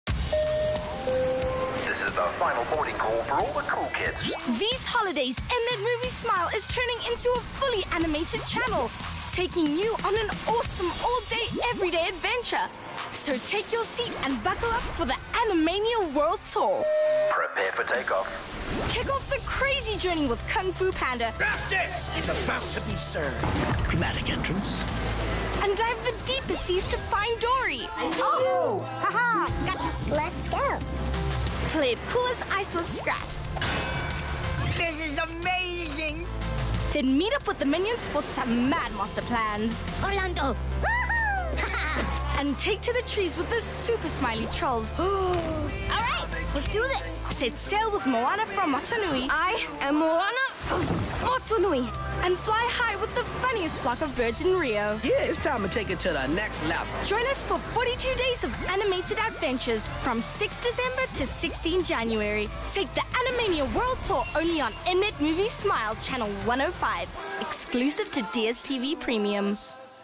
Animania World Tour on DStv
Take the Animania World Tour all day everyday with M-Net Movies Smile on DStv Channel 105 from 6 December - 16 January. Exclusive to DStv Premium